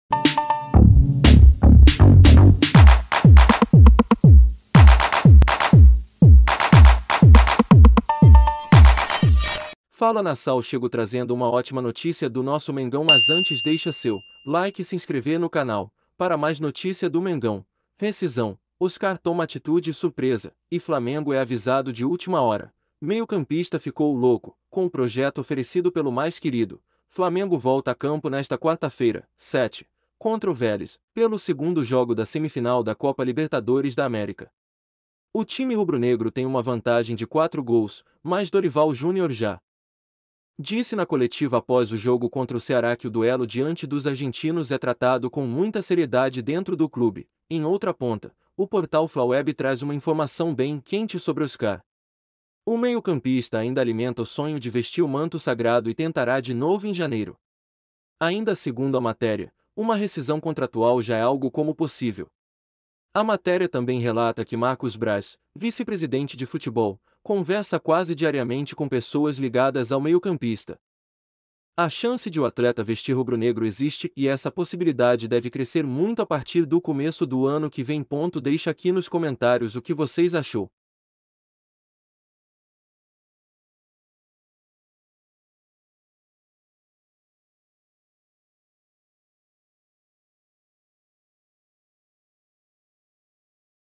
URGENTE ACABOU DE CONFIMA BRAZ AGIU NA SURDINA UTIMAS NOTÍCIAS DO FLAMENGO
FALA NAÇAO CHEGO TRAZENDO UMA ÓTIMA NOTÍCIA DO NOSSO MENGÃO MAS ANTES DEIXA SEU
PARA MAS NOTÍCIA DO MENGÃO
Rescisão”; Oscar toma atitude ‘surpresa’ e Flamengo é avisado de última hora
Meio-campista ficou "louco" com o projeto oferecido pelo Mais Querido
 Flamengo volta a campo nesta quarta-feira (7), contra o Vélez, pelo segundo jogo da semifinal da Copa Libertadores da América